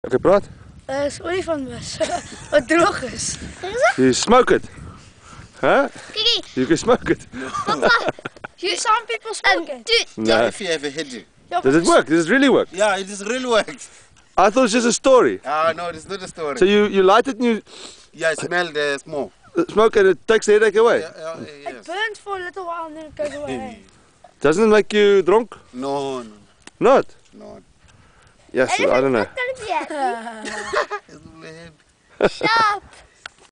Elephant Dung cures headaches.
This guide explains that Elephant dung cures headaches. This is in the Kruger National Park, near Pretoriuskop.

I specialize in personal attention and make your Tour unforgettable! for more detail.